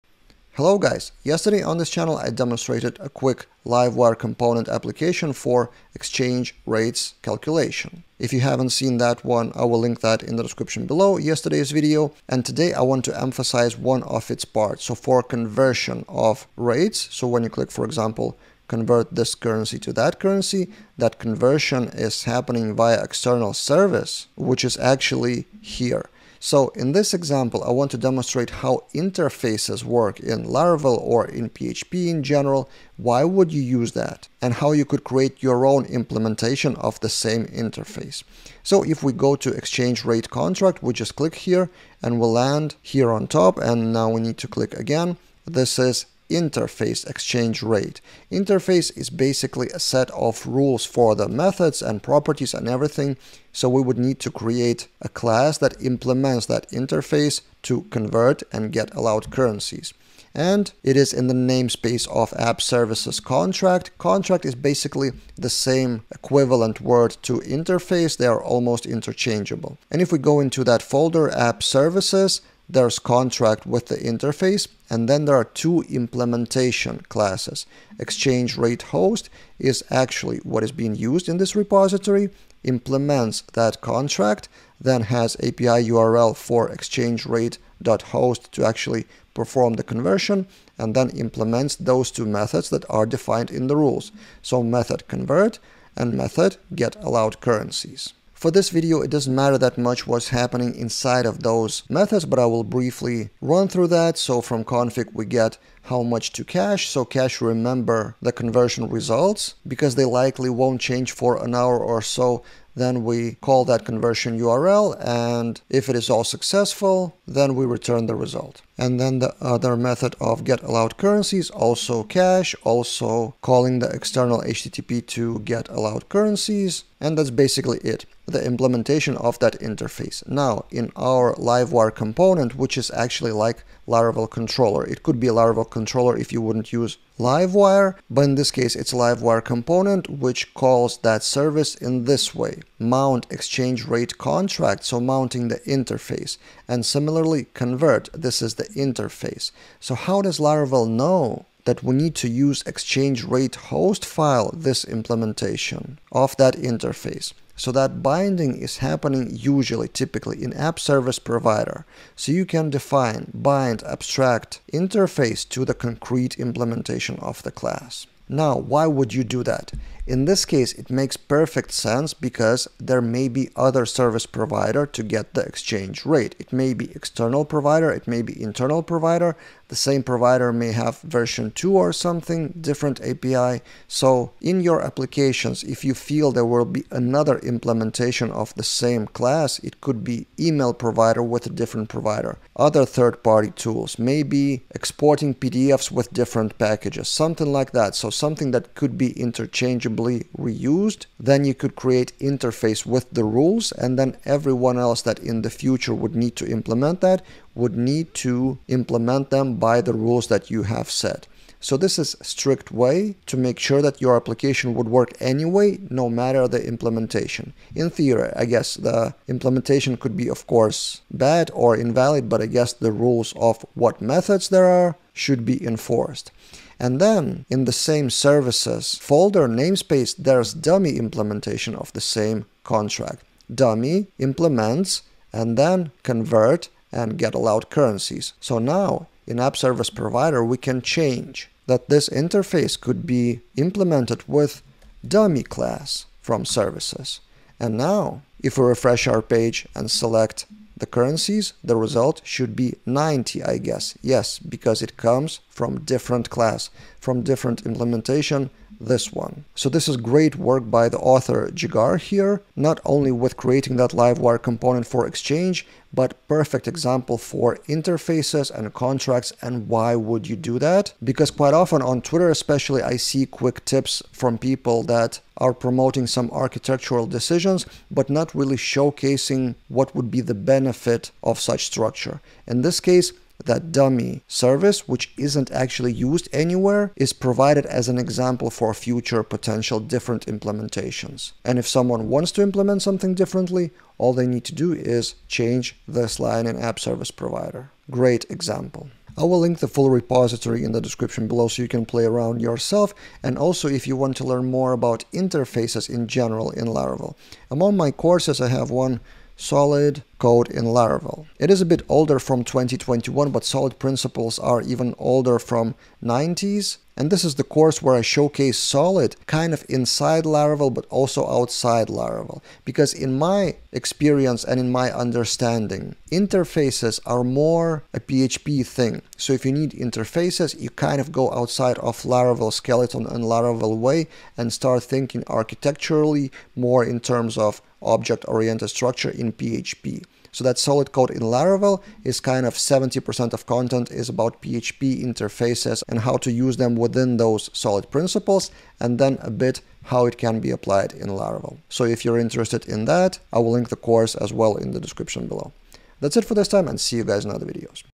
Interfaces in Laravel: Practical Example of Exchange Rates
How to use Interfaces (or Contracts) in Laravel and WHY would you want to do it? A practical example.